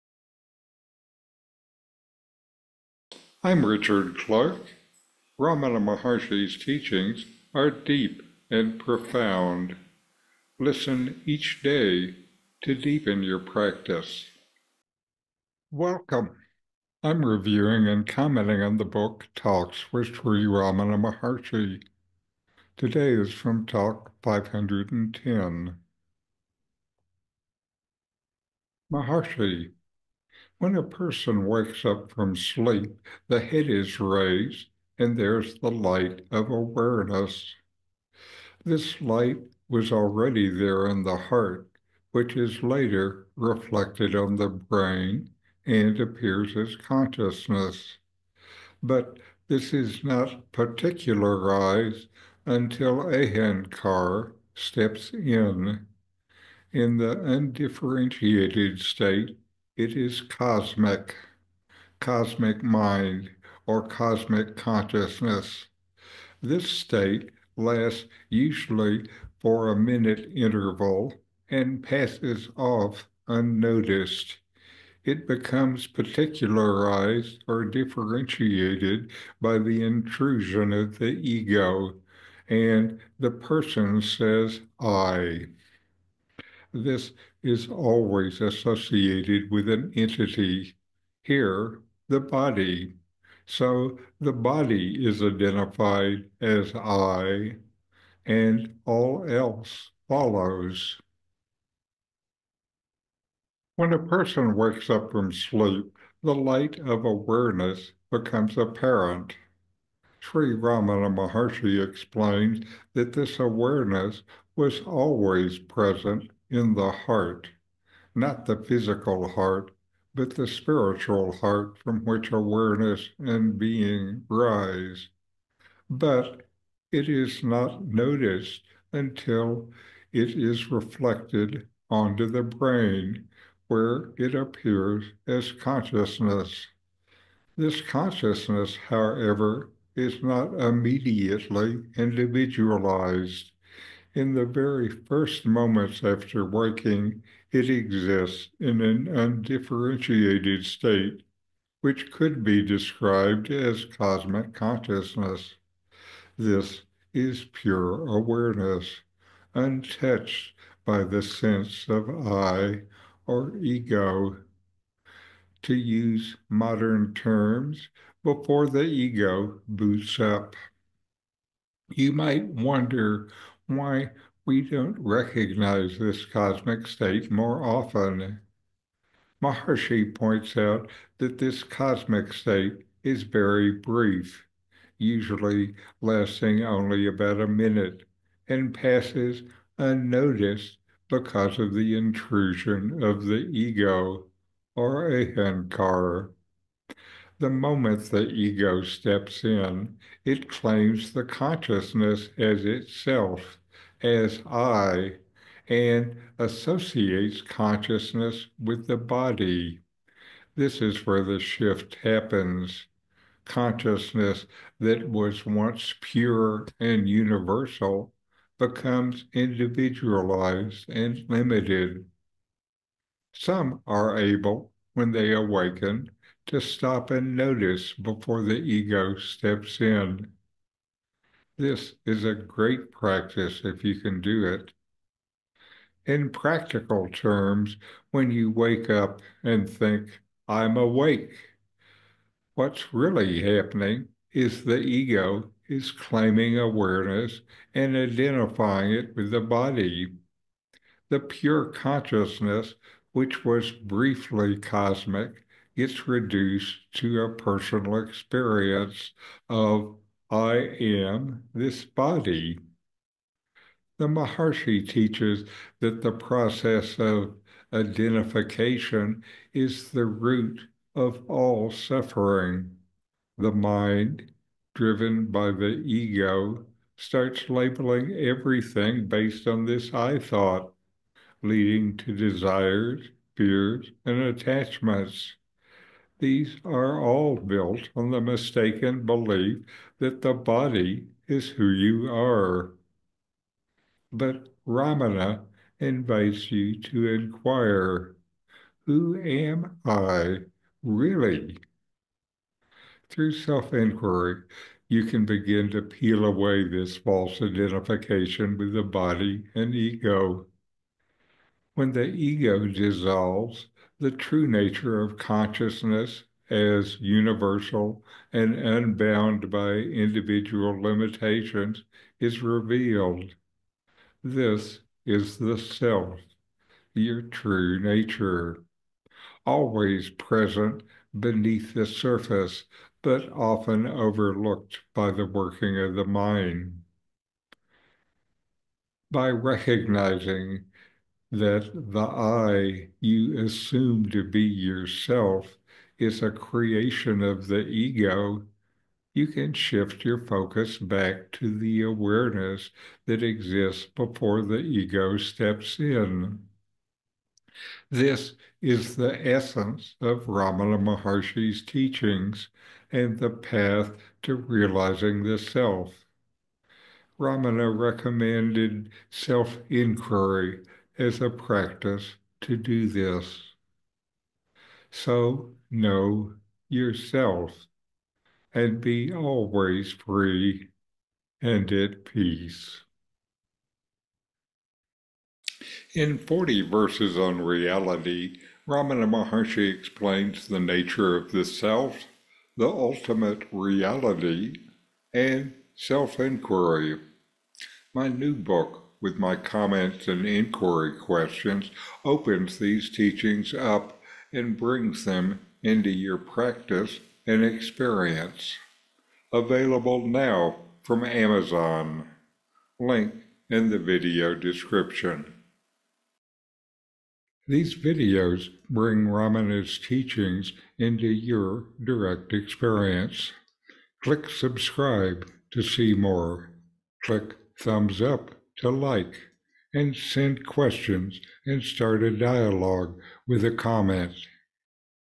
When you first awaken - Ramana Clips Talk # 510
Ramana Maharshi: When a person wakes up from sleep the head is raised and there is the light of awareness. This light was already there in the heart which is later reflected on the brain and appears as consciousness. But this is not particularized until ahankar steps in. In the undifferentiated state it is cosmic (cosmic mind or cosmic consciousness). This state lasts usually for a minute interval and passes off unnoticed. It becomes particularized or differentiated by the intrusion of the ego and the person says ‘I’. This is always associated with an entity (here, the body). So the body is identified as ‘I’ and all else follows.

Comments by Richard Clarke. From "Talks with Sri Ramana Maharshi."
My book “Forty Verses on Reality by Ramana Maharshi with comments” is available from Amazon.
It is free with Kindle Unlimited.